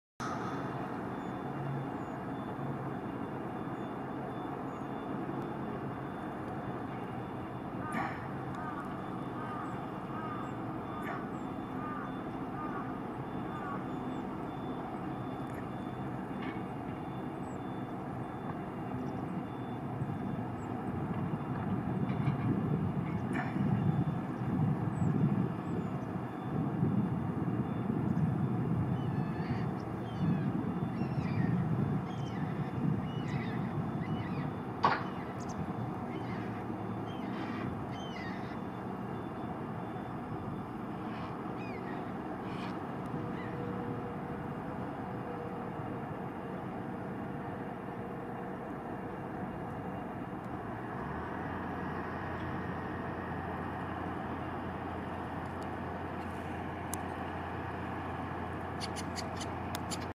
ツグミの喧嘩＠宮城県岩沼市 Battle of Dusky Thrush
ツグミ2羽が喧嘩をしているところです。
（平成28年12月7日、宮城県岩沼市で撮影）

This is the battle of two Dusky Thrushes(Turdus naumanni).
I was shooting this video at Iwanuma city,Miyagi prefecture,Japan on December 7,2016.